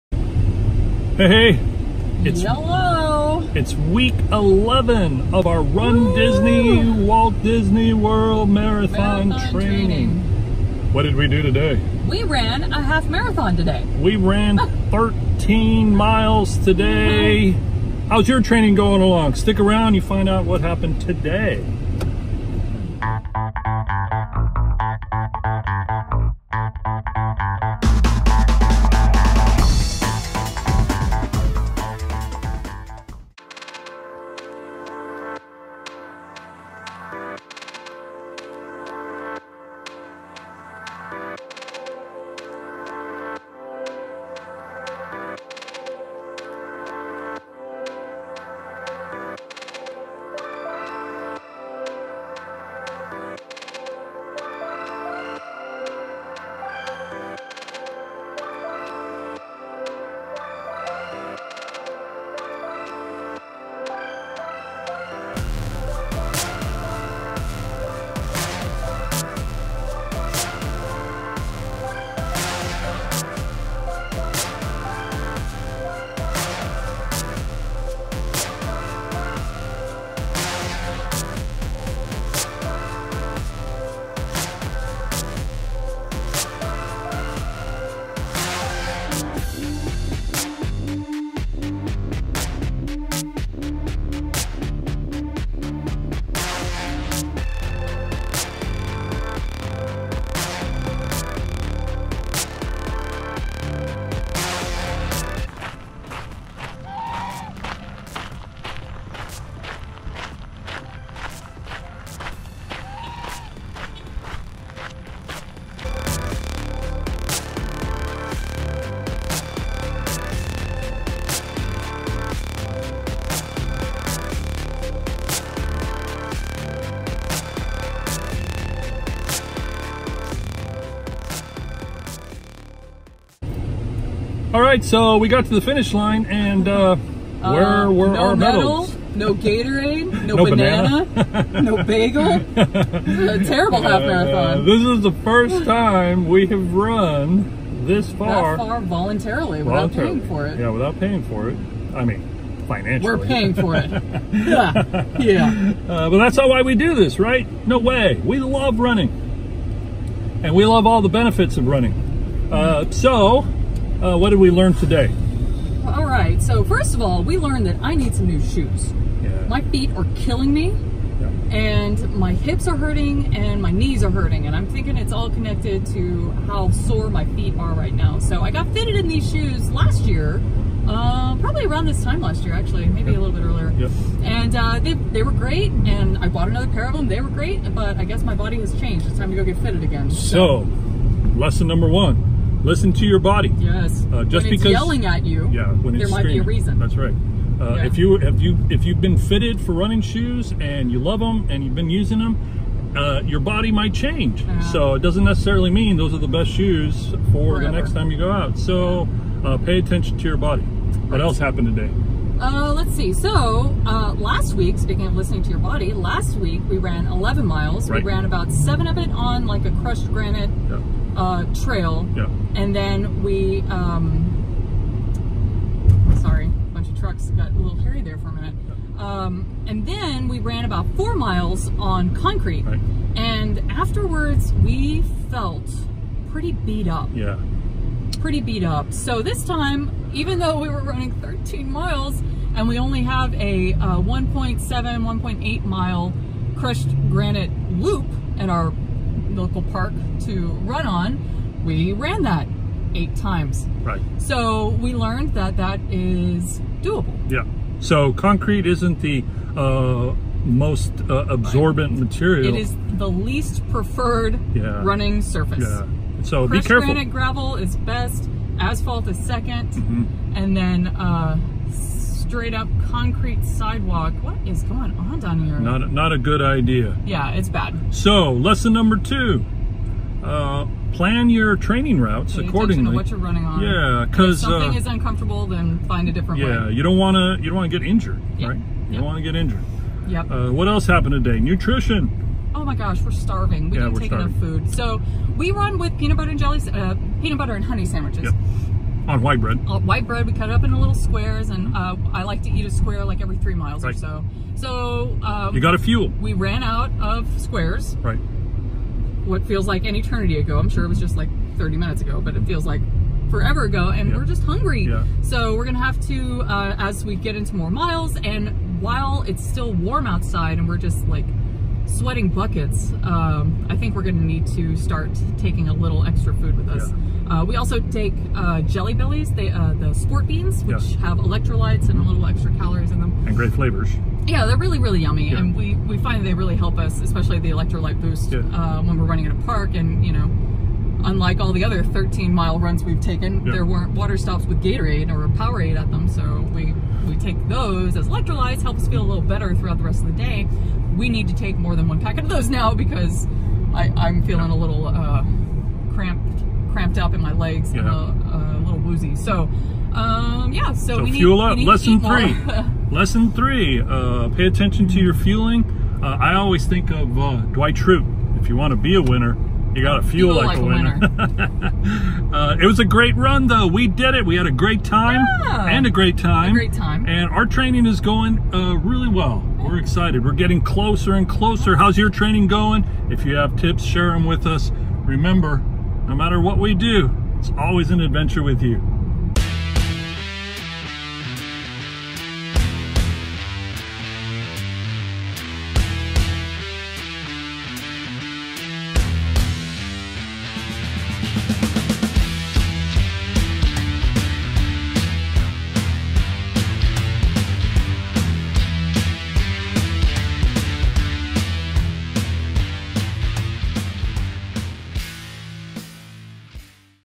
RunDisney Marathon Training Week 11
This weekend we hit a big milestone - it was the FIRST time we've EVER run 13 miles...voluntarily. No race registration, no butterflies leading up to the big day, no bibs, no medals. We just *went* for a 13 mile run. And we realized, from here on out, every long run will be taking us further than we've ever run before.

If we're honest, we feel pretty darn good about that. 11 weeks down, 17 to go!